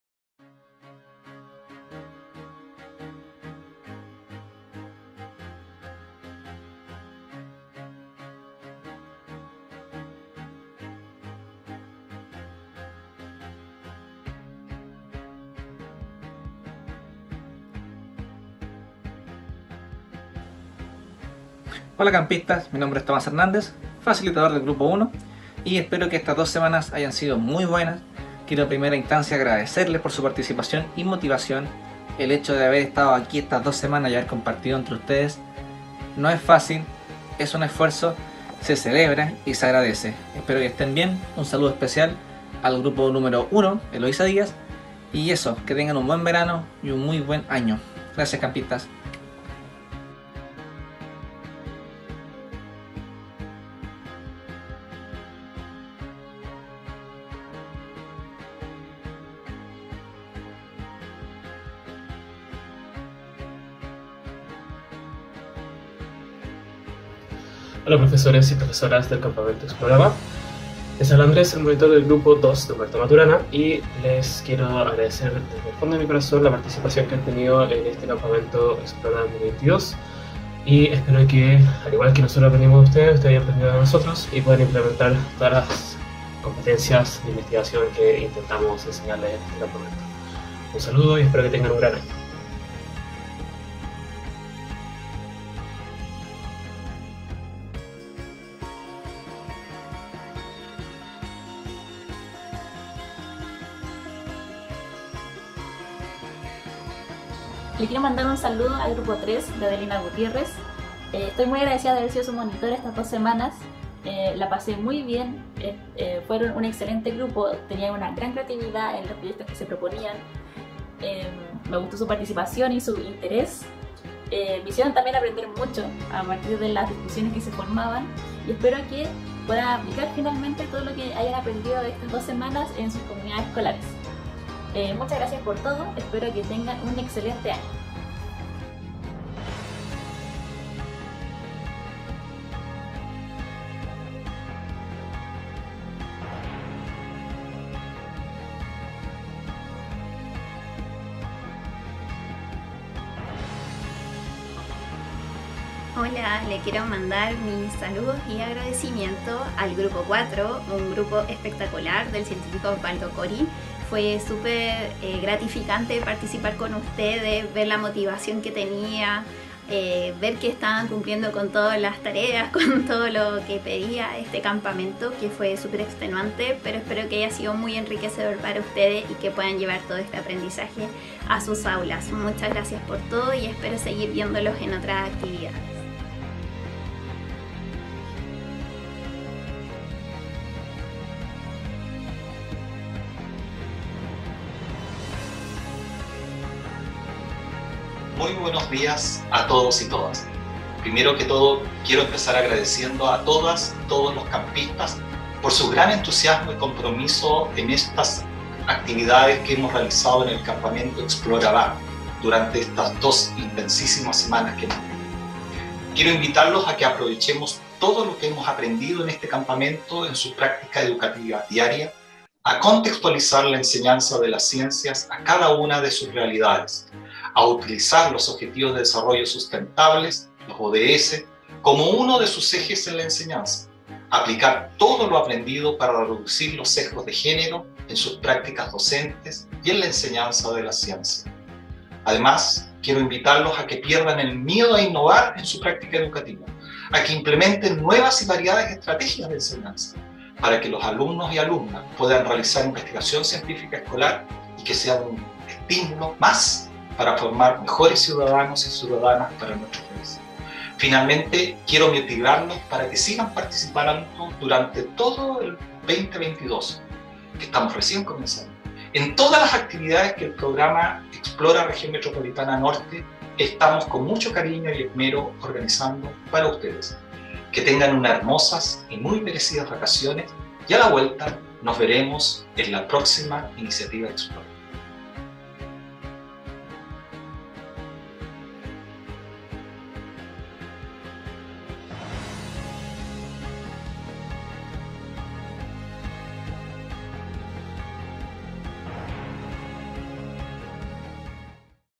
🥳🎉No es un adiós, es un ¡Bienvenidos a nuestra comunidad ExploraRMNorte!
Durante las dos primeras semanas de enero, 28 docentes de educación básica y media de diversas disciplinas, junto a educadoras/res de párvulo de la RegionMetropolitana, se reunieron por primera vez virtualmente en el Campamento Explora VA! ExplroaRMNORT“Construyendo comunidades de aprendizaje en CTCI”.

A través de actividades sincrónicas y asincrónicas los profesores realizaron diversas dinámicas con el fin de promover la apropiación de herramientas pedagógicas, basadas en el modelo de competencias e impulsar la formación de comunidades de aprendizaje y redes de colaboración entre los docentes

¡Esperamos el próximo año a muchos profes más! ❤️❤️